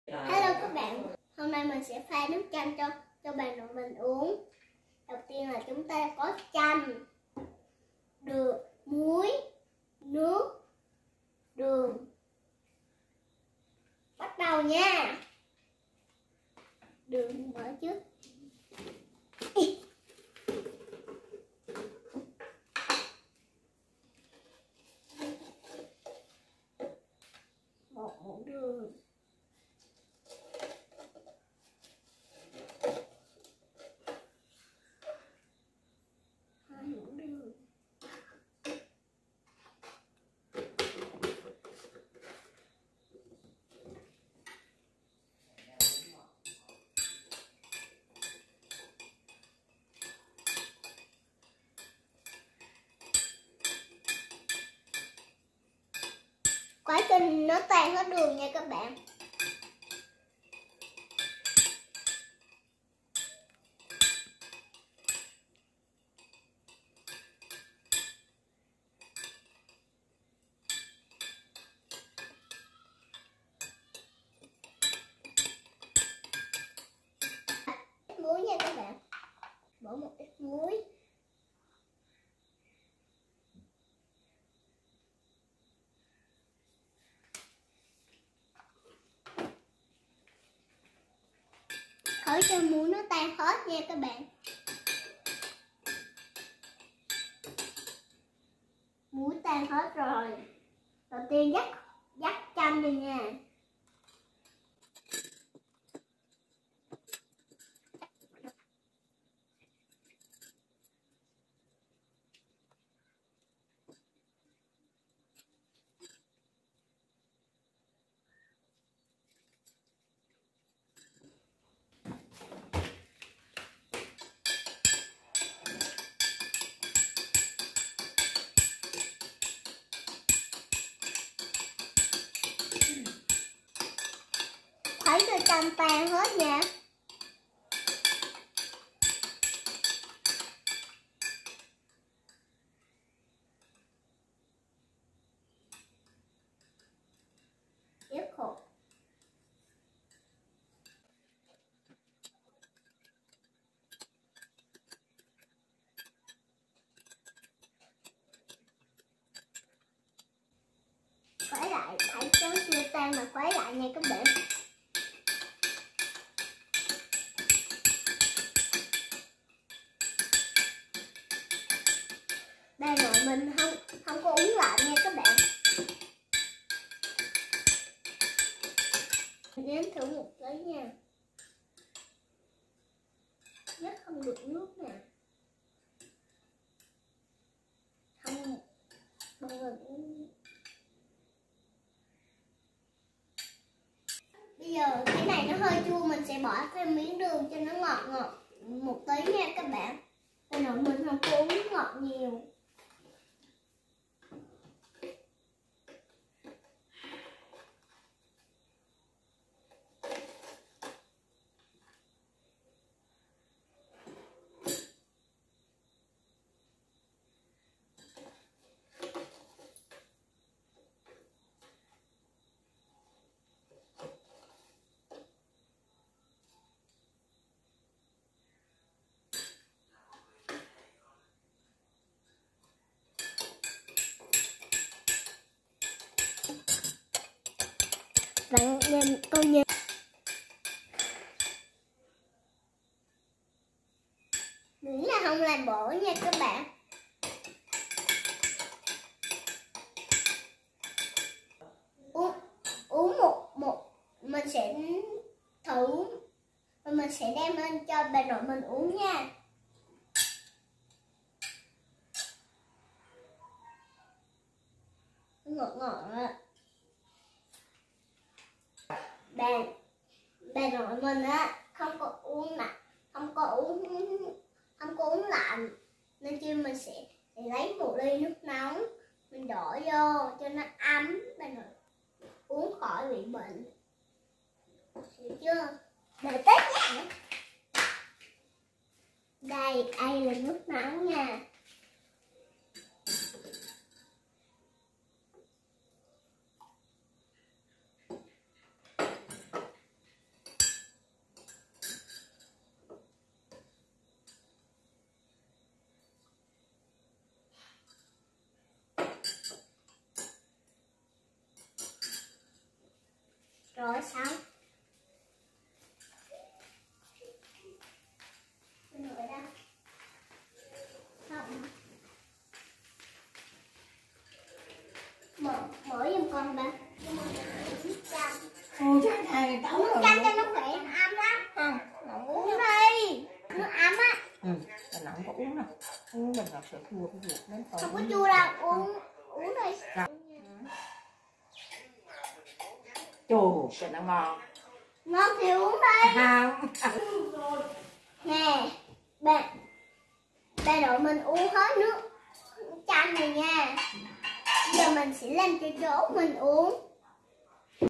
Cách Làm Nước Chanh // Nước Chanh Boy Học Từ Cô Giáo
Con trai tự làm nước chanh cho bà nội uống lúc bị bệnh sau khi học cách làm từ cô giáo. Nước chanh boy làm tuy đơn giản nhưng đặc cả tâm huyết vào đây. Ly nước chanh này thật đặc biệt